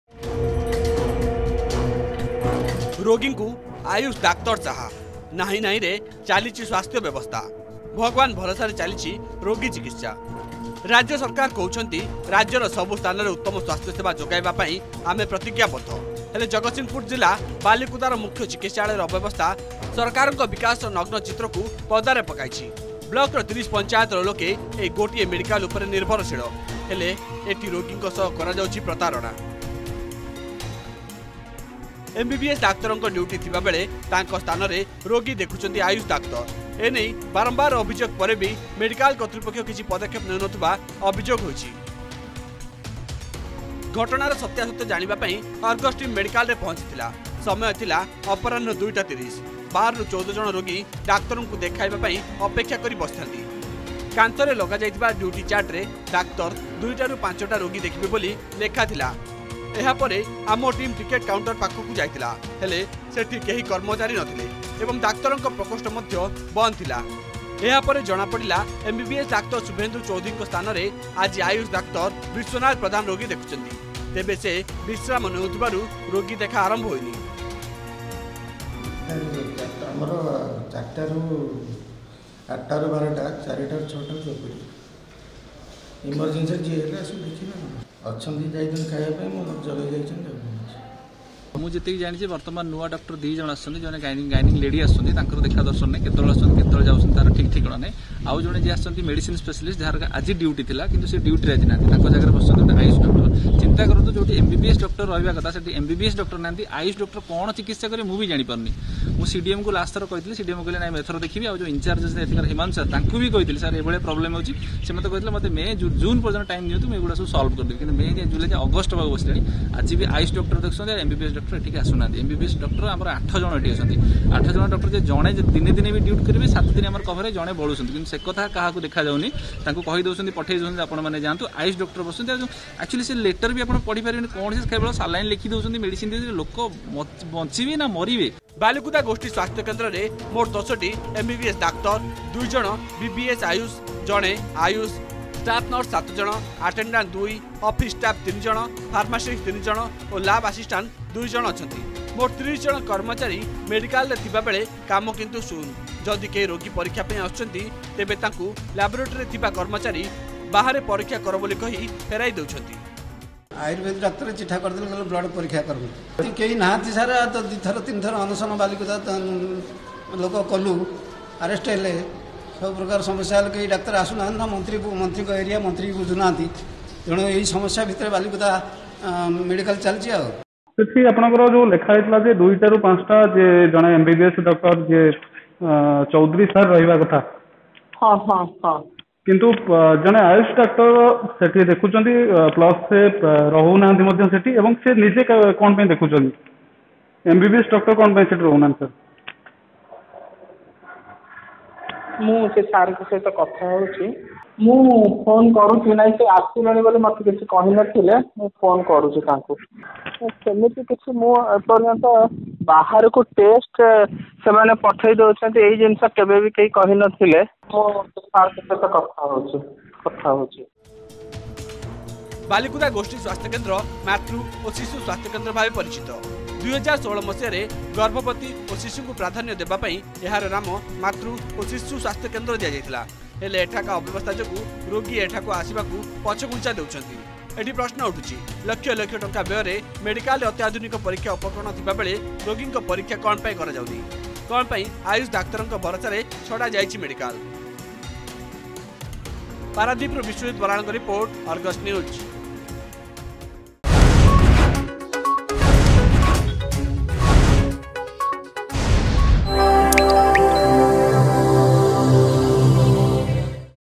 ଭଗବାନ ଭରସାରେ ଚାଲିଛି ରୋଗୀ ଚିକିତ୍ସା
ବାଲିକୁଦା ଗୋଷ୍ଠୀ ସ୍ୱାସ୍ଥ୍ୟ କେନ୍ଦ୍ରରେ ବିପର୍ଯ୍ୟୟ ସ୍ୱାସ୍ଥ୍ୟ ବ୍ୟବସ୍ଥା । ଏମବିବିଏସ ଡାକ୍ତରଙ୍କ ସ୍ଥାନରେ ରୋଗୀ ଦେଖୁଛନ୍ତି ଆୟୁଷ ଡାକ୍ତର । ଡାକ୍ତରୀ ପରୀକ୍ଷା ପାଇଁ ଉପକରଣ ଥିଲେ ବି ଲୋକଙ୍କୁ ବାହାରେ ପରୀକ୍ଷା କରିବାକୁ କହୁଛନ୍ତି ମେଡିକାଲ କର୍ମଚାରୀ ।